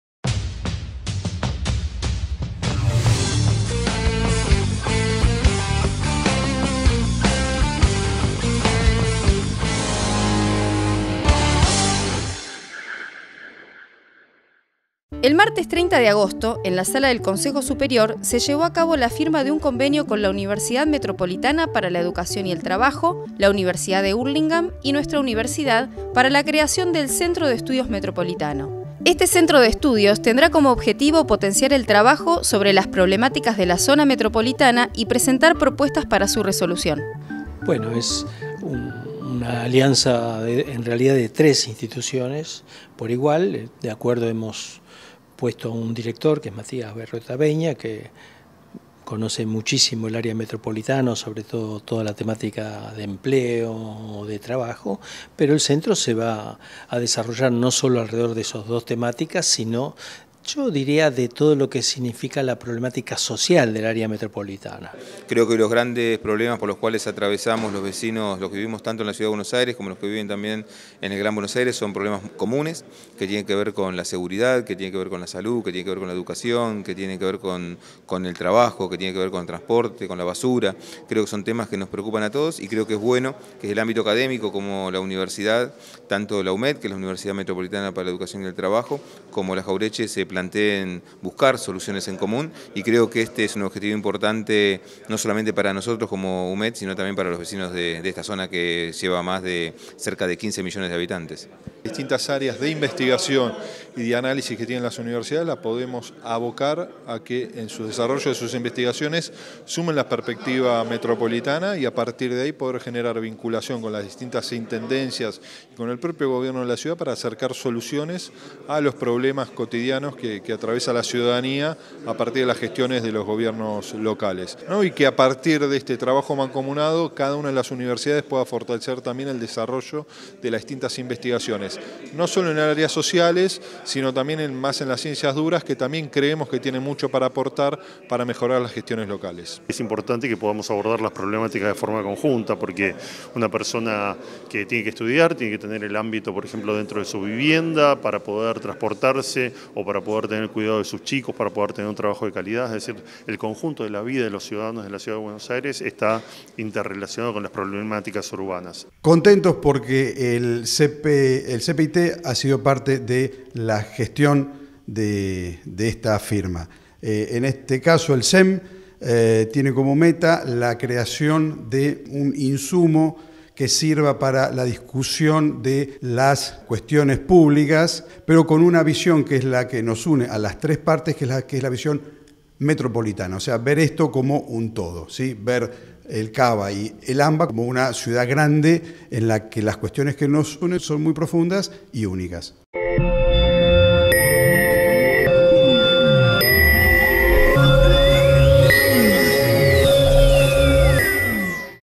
Convenio para la creación del Centro de Estudios Metropolitano
El martes 30 de agosto en la Sala del Consejo Superior;  se llevo a cabo la firma de un convenio con la Universidad Metropolitana para la Educación y el Trabajo (UMET); la Universidad de Hurlingham y nuestra universidad; para la creación del Centro de Estudios Metropolitano. Este centro de estudios tendrá como objetivo potenciar el trabajo sobre las problemáticas de la zona metropolitana y presentar propuestas para su resolución.
gida a generar y desarrollar proyectos basados en Tecnologías de la Información y la comunicación.